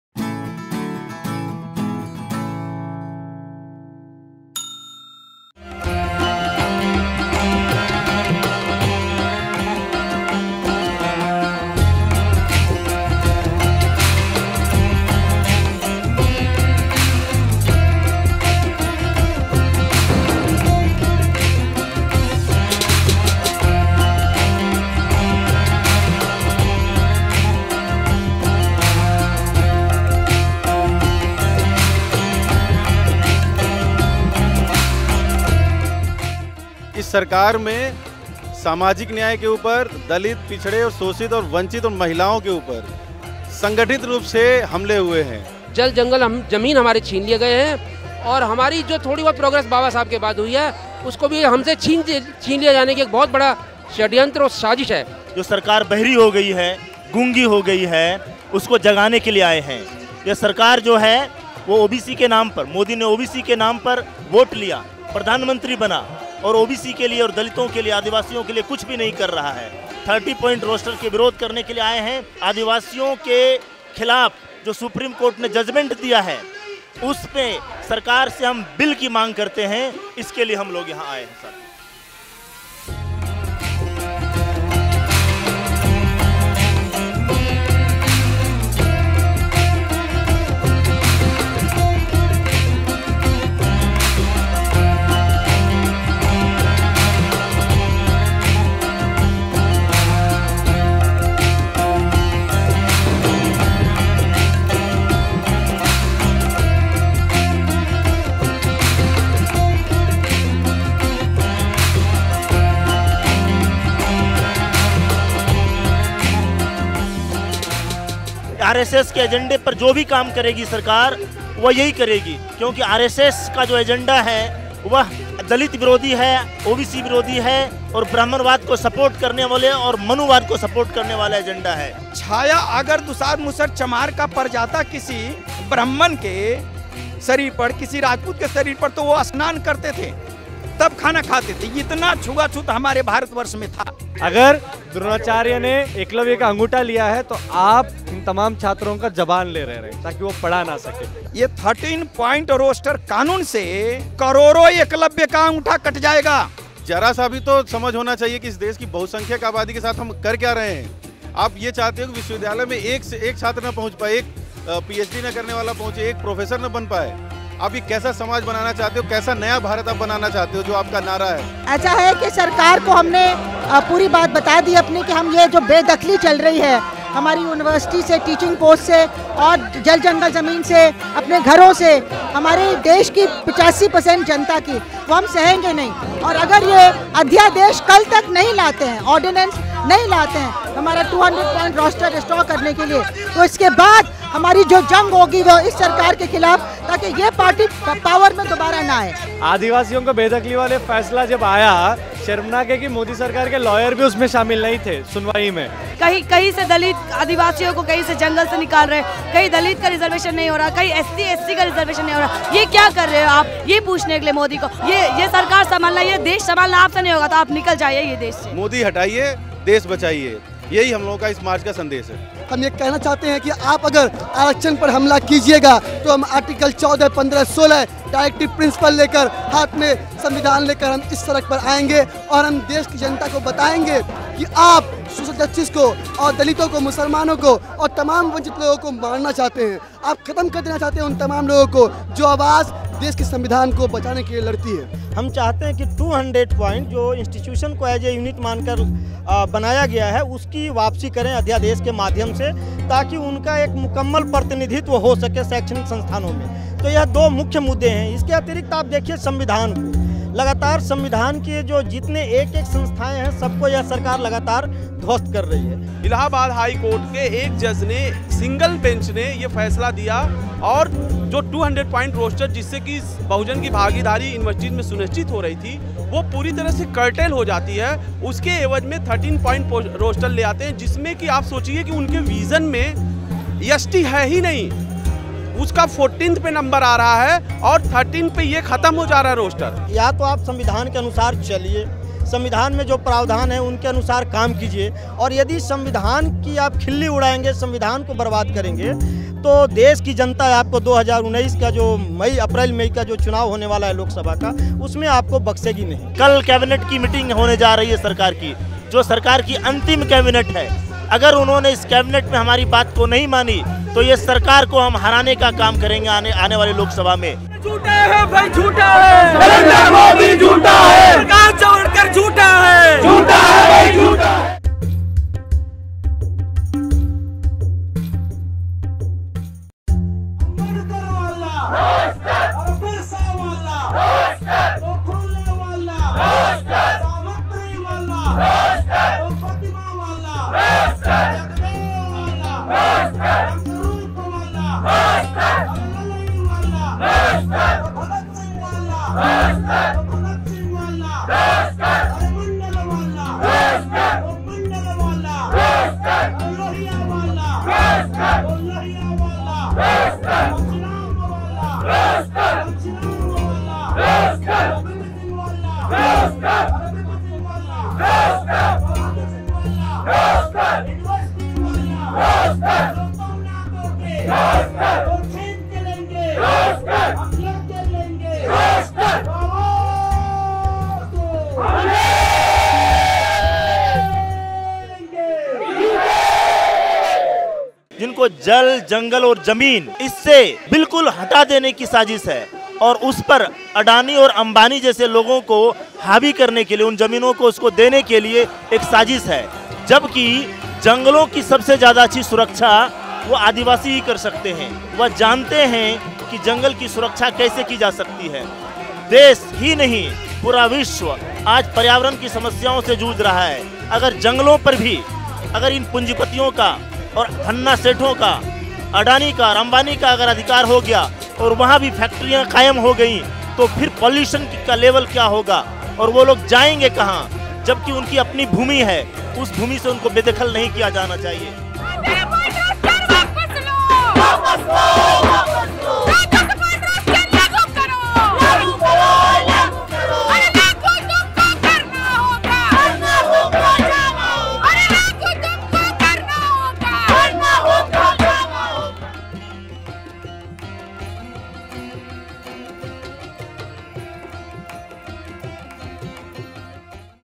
Forced Out of Homes and Universities - Adivasis Under Attack
On March 5, Adivasi and Dalit groups, teachers' organisations and political parties across India called for 'Bharat Bandh' against the recent Supreme Court order on eviction of lakhs of Tribals and Forest Dwellers from their habitat. The Bandh was supported by various national leaders, activists and organisations.
Hundreds of teachers under the banner of Delhi University Teacher's Association marched to Parliament street on 5th March in solidarity with the Bandh and also rejecting the 13 point roster system.